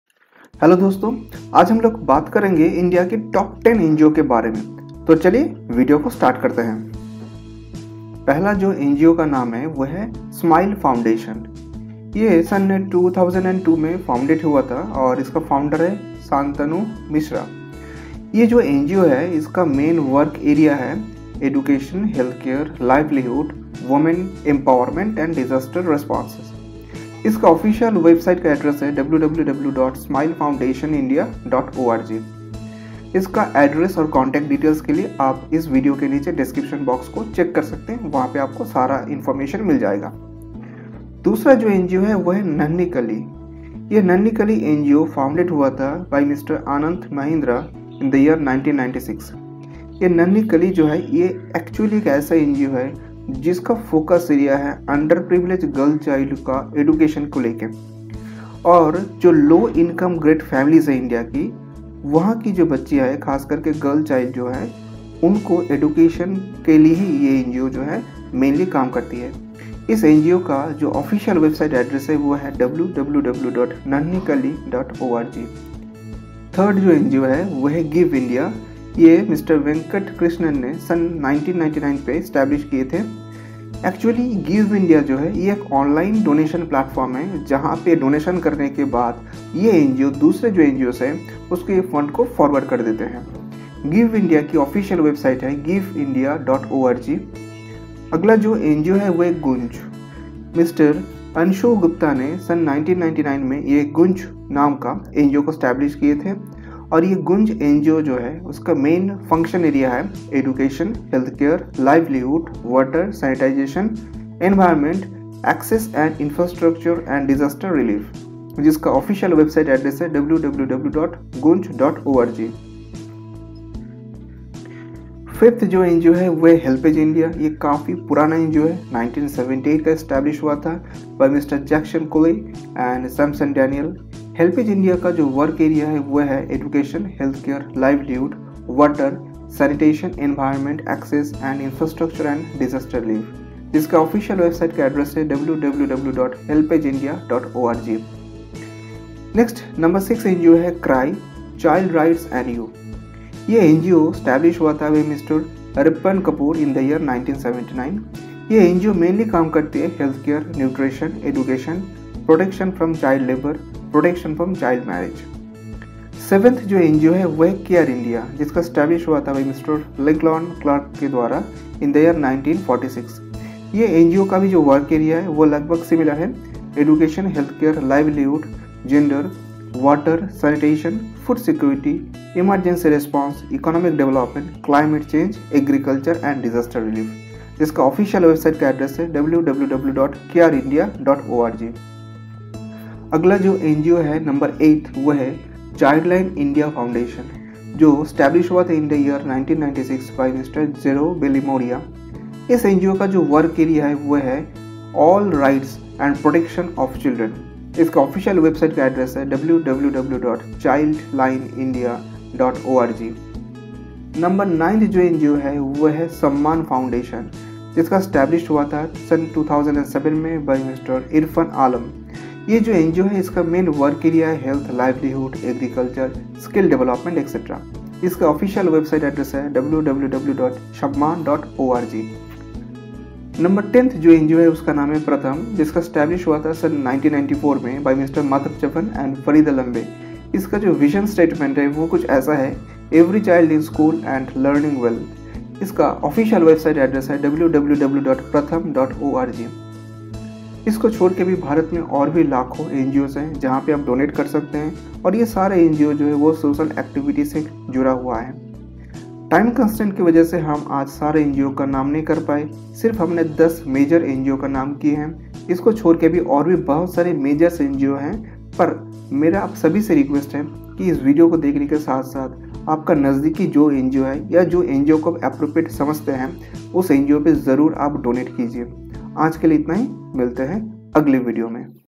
Top 10 NGO In India :: 2022 ::
1. SMILE FOUNDATION Non-Governmental Organization 2002 Santanu Mishra We work on Education, Healthcare, Livelihood, Women Empowerment and Disaster response.
2. NANHI KALI Non-Governmental Organization 1996 Anand Mahindra Project Nanhi Kali, which translates to ‘a little bud’ in Hindi, supports the education of underprivileged girls in India. Designed to help girls from low-income families complete ten years of formal schooling, the project has impacted the lives of over 450,000 girls (called Nanhi Kalis) from underserved communities across the country.
3. GIVEINDIA Non-Profit Organisation
1999
Venkat Krishnan GiveIndia, an online donation platform, allows you to support a cause of your choice from the NGOs evaluated for transparency and credibility.
4. GOONJ Non-Governmental Organization 1999
Anshu Gupta
 We work on Education, Healthcare, Livelihood, Water, Sanitation, Environment, Access & Infrastructure and Disaster relief.

5. HELP AGE INDIA Non-Governmental Organization 1978
Mr Jackson Cole, Samson Daniel
6. CRY
(CHILD RIGHTS AND YOU) Non-Governmental Organisation 1979 Rippan Kapur Healthcare
 Nutrition
Education
Protection from child labour
Protection from child marriage CRY – Child Rights and You, 189/A Anand Estate, Sane Guruji Marg, Mumbai 400 011

7. CARE INDIA Non-Governmental Organization 1946 Lincoln Clark We work on Education, Healthcare, Livelihood, Gender, Water, Sanitation, Food Security, Emergency Response, Economic Development, Climate Change, Agriculture and Disaster relief. A-12, Bhilwara Towers, Third Floor, Tower-II, Sector-1, Noida, Gautam Buddha Nagar,
8..
CHILDLINE INDIA FOUNDATION
 Non-Governmental Organization
1996

Jeroo Billimoria
We all work for the rights and protection of children. B-1101, 11th Floor, Ratan Central, Dr Babasaheb Ambedkar Road, Parel East, Mumbai 400 012, Maharashtra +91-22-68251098
9.
SAMMAN FOUNDATION
 Not For Profit 2007
Irfan Alam
 We work in the fields of Health, Livelihood, Agriculture, Skill Development etc.
 2/30, SBI Colony, Khazpura, Near Jagdeo Path, Bailey Road, Patna-800014

10. PRATHAM Non-Governmental Organization 1994 Madhav Chavan, Farida Lambay
“Every Child in School and Learning well”
Delhi Registered Office B- 4/58, Safdarjung Enclave 2nd Floor, New Delhi – 110 029